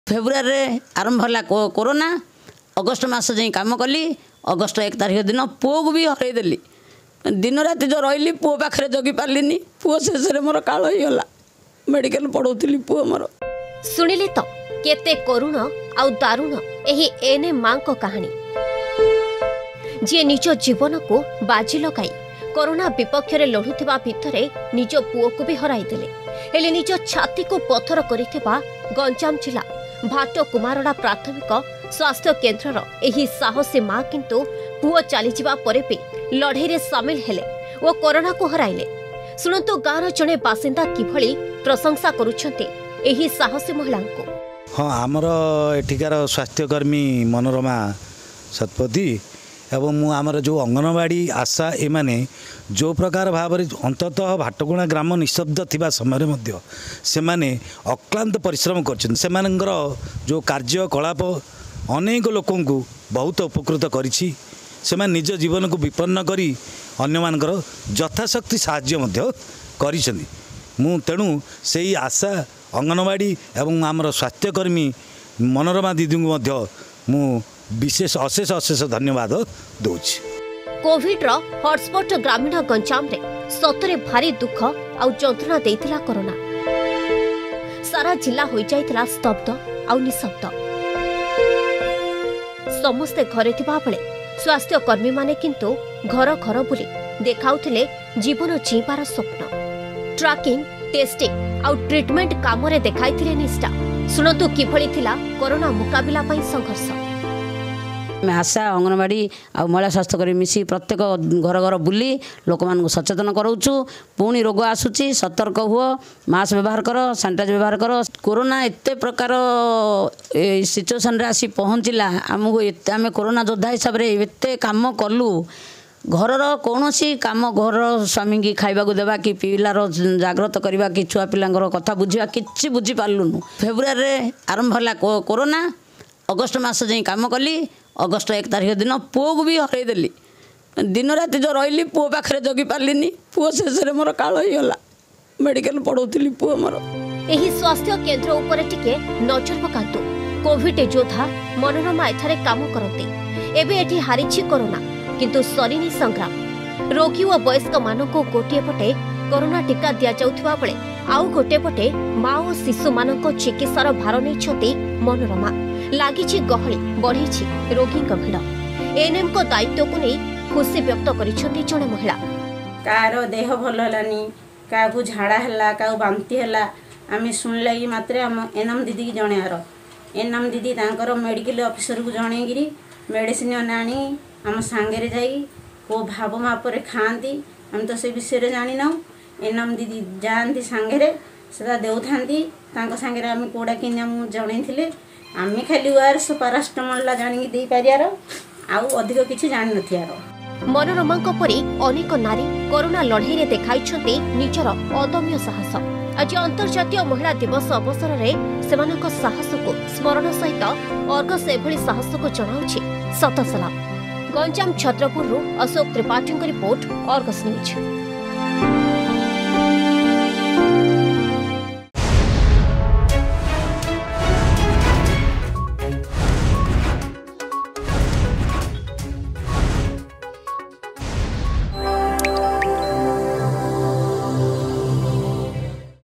କୋଭିଡ ଯୁଦ୍ଧରେ ସଂଗ୍ରାମ କରୁଥିବା ବେଳେ ଘରେ ତେଣେ ତାଙ୍କର ପୁଅ ଚାଲିଯାଇଥିଲେ ପରପାରିକୁ ...
ନିଜ ଜୀବନକୁ ବାଜି ଲଗାଇ କରୋନା ବିପକ୍ଷରେ  ଲଢ଼ୁଥିବା ଭିତରେ ନିଜ ପୁଅକୁ ବି ହରାଇଦେଲେ । ହେଲେ ନିଜ ଛାତିକୁ ପଥର କରିଥିବା ଗଂଜାମ ଜିଲ୍ଲା ଭାଟକୁମାରଡା ପ୍ରାଥମିକ ସ୍ୱାସ୍ଥ୍ୟକେନ୍ଦ୍ର ର ଏହି ସାହସୀ ମା କିନ୍ତୁ ପୁଅ ଚାଲିଯିବା ପରେ ବି ଲଢେଇ ରେ ସାମିଲ ହେଲେ ଓ କରୋନା କୁ ହରାଇଲେ...